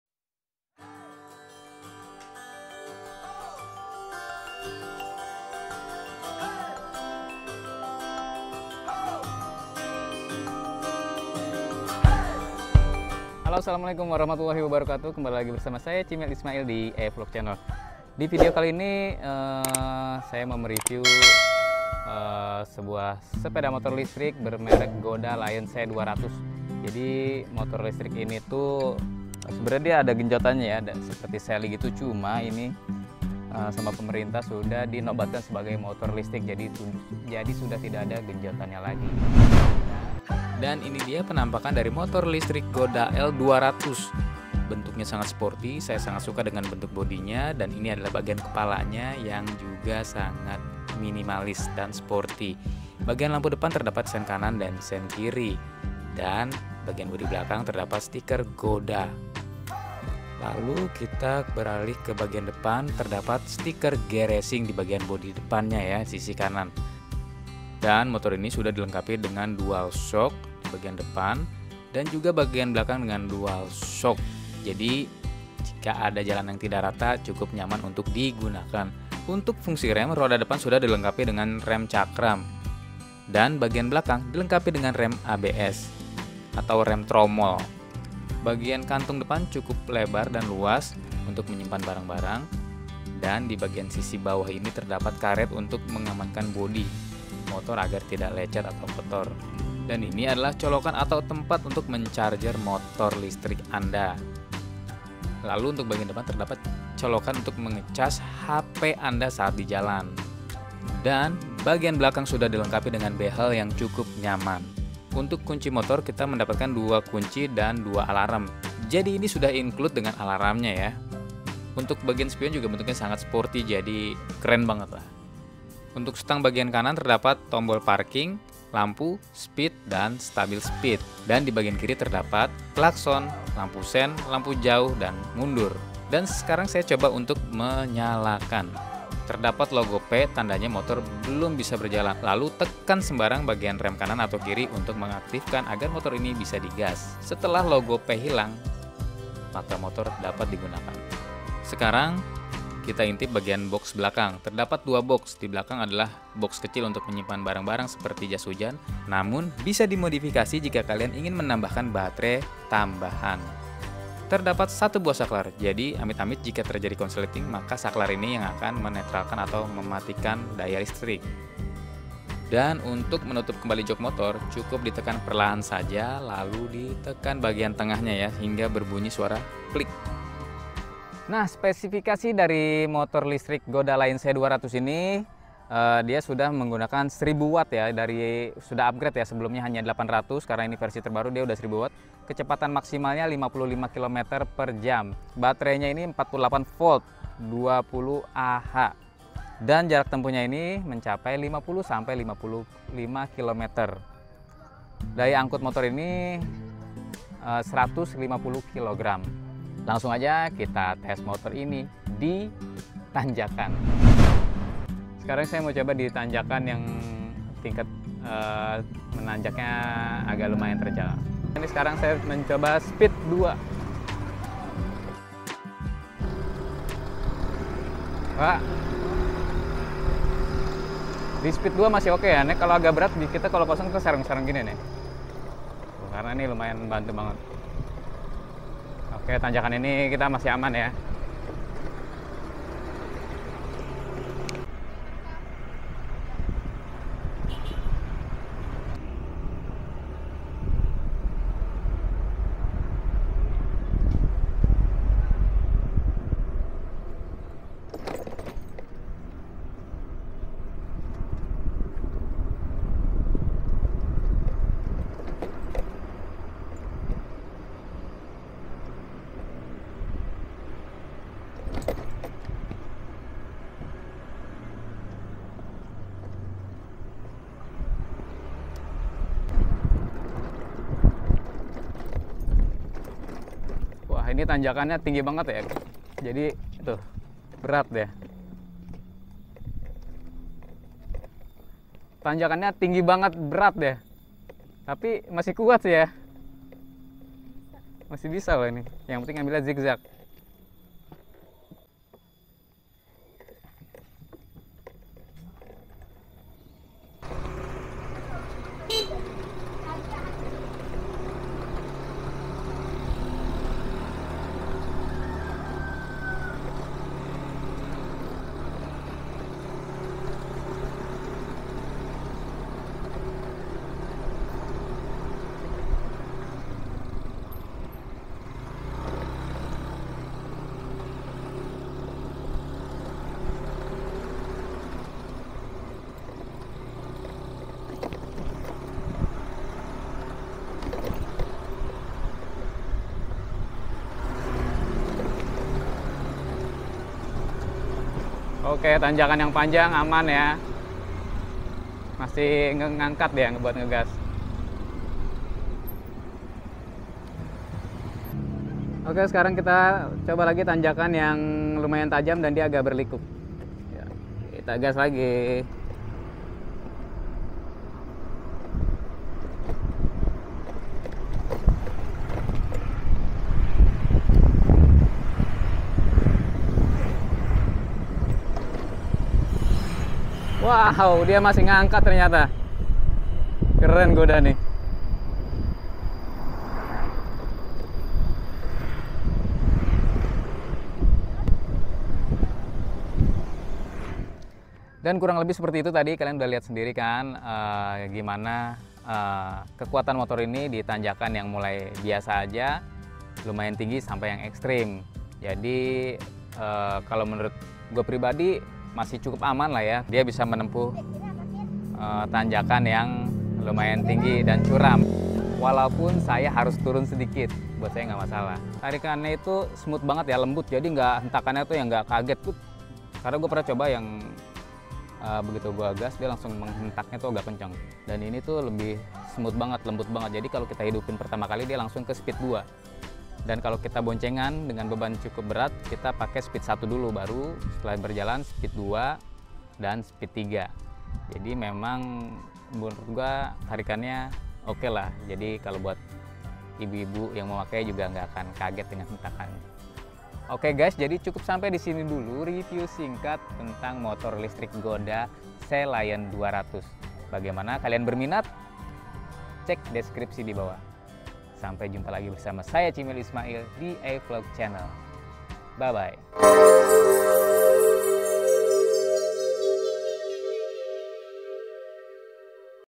TEST MOTOR LISTRIK GODA GOLDEN NEW L 200 CUMA 1 JUTAAN
Halo teman teman kali ini saya mau review dan test motor listrik godal200, langsung aja cekidoot.